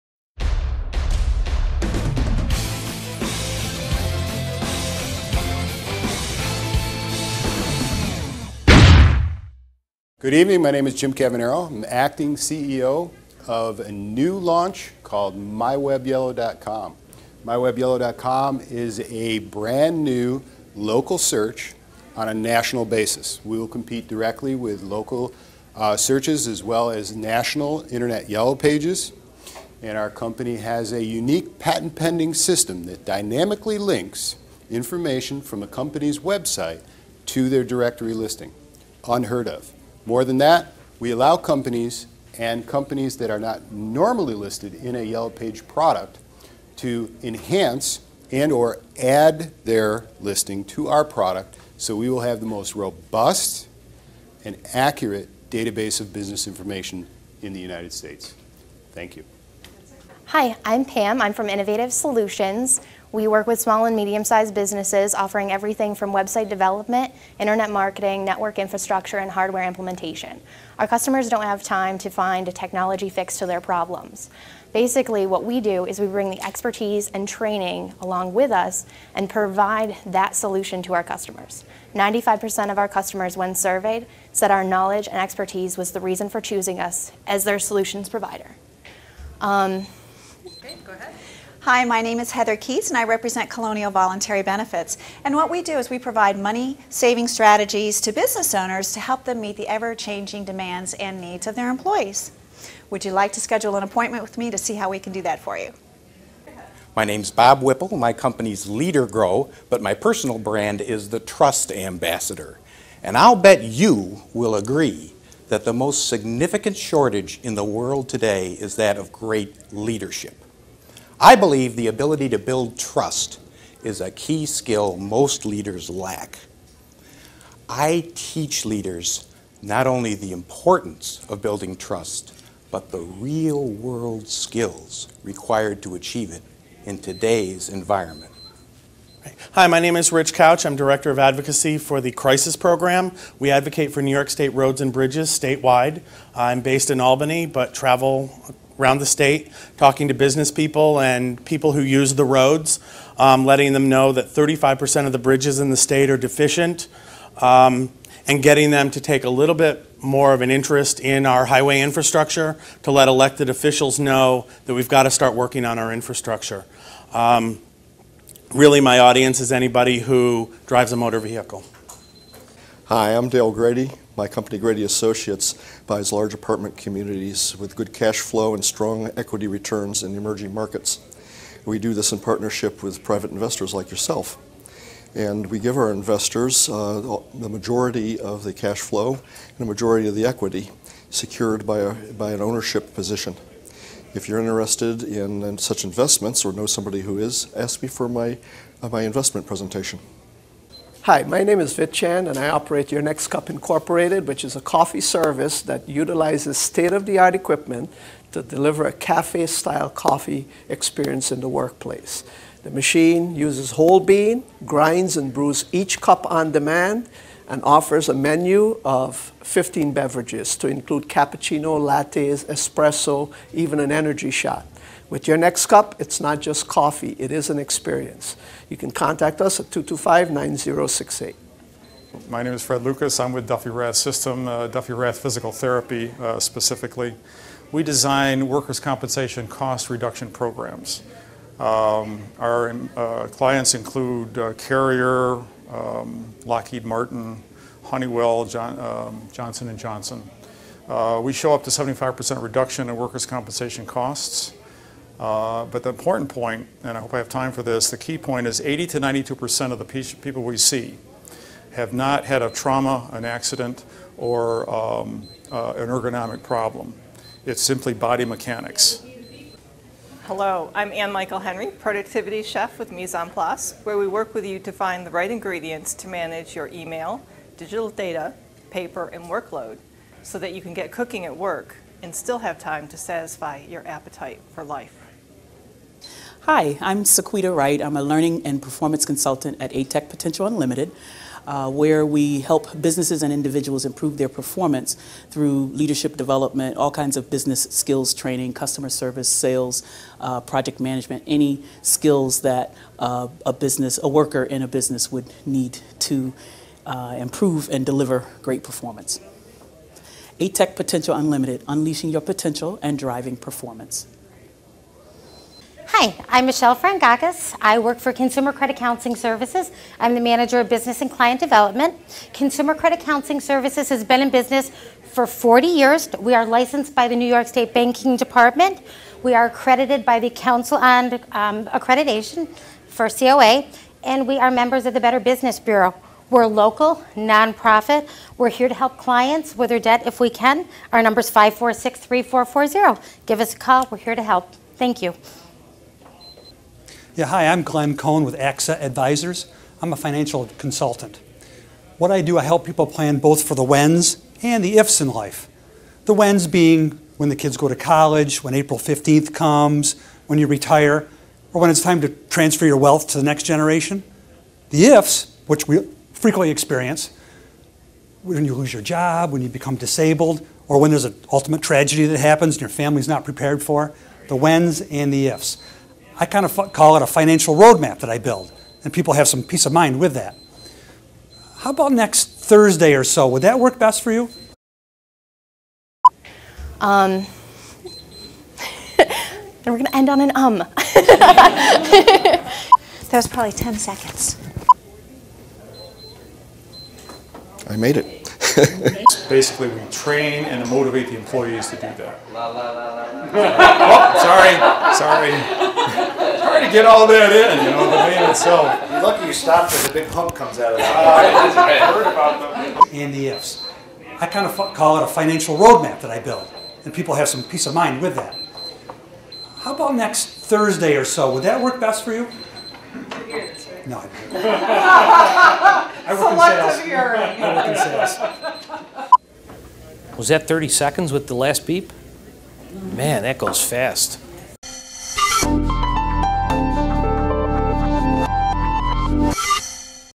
BAHandElevatorPitches
Our Business After Hours event August 6 was a dual event with general networking in one room and the "Elevator Pitch" contest in another room.  This video includes footage of networking, all contestants (besides the winner) and bloopers.